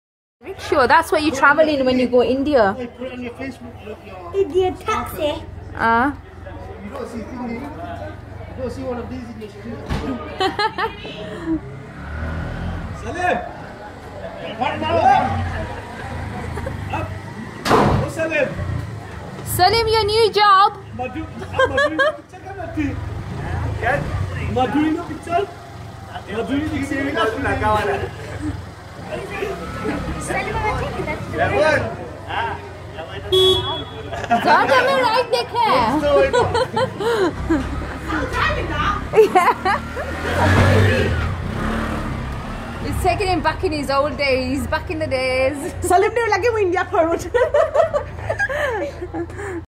Indian Auto Riksha in UK..
People in UK came out on the road when they saw Indian auto first time..